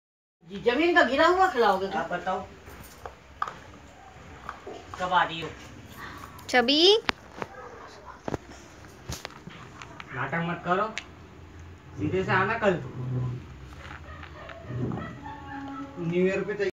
Saint bernard eating pedigree | 1 month old saint bernard
This is bruno - the st bernard. He is 1 month old cute st bernard. This is bruno's very 1st video when we had bought him home & made him feed for the first time. he js the cutest st bernard. this is indian st bernard.